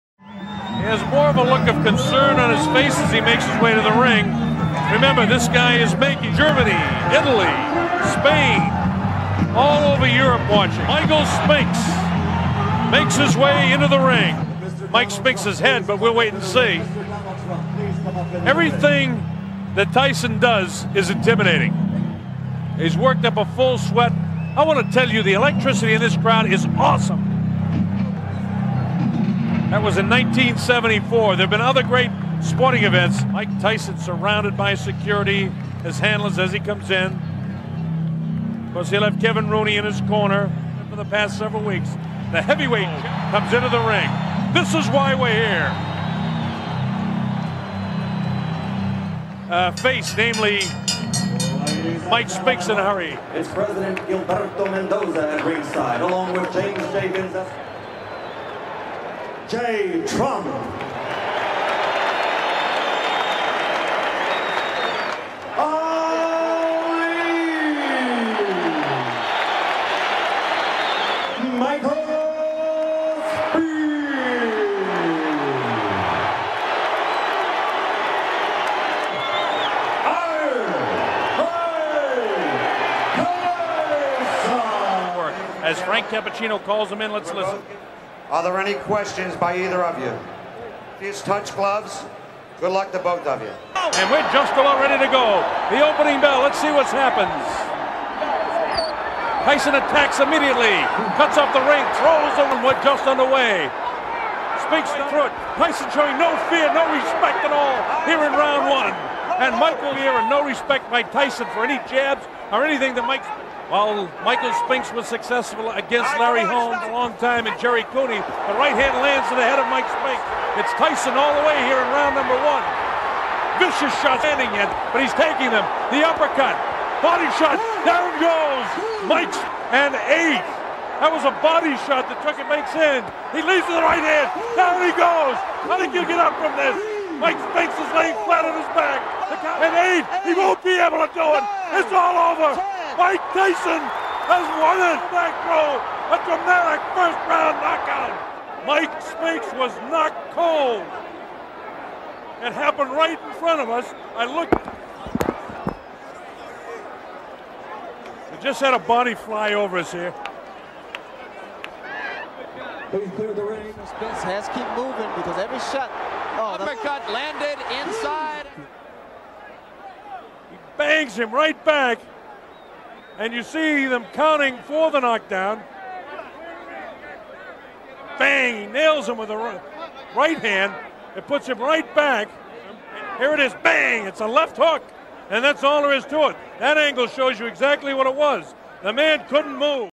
Mike Tyson (USA) vs Michael Spinks (USA) | KNOCKOUT, BOXING fight, HD
Professional american heavyweight boxer Mike Tyson with nickname "Iron" (34–0, 30 KO - pre-fight record) against champion in two weight classes and olympic gold medalist 1976 Michael Spinks (31–0, 21 KO - pre-fight record). Both were undefeated and each competed for the title of the legal heavyweight champion. Fight took place at Convention Hall in Atlantic City, USA on June 27, 1988. Spectacular boxing fight, HD.
Mike Tyson (USA) vs Michael Spinks (USA) | KNOCKOUT, BOXING fight, HD HIGHLIGHTS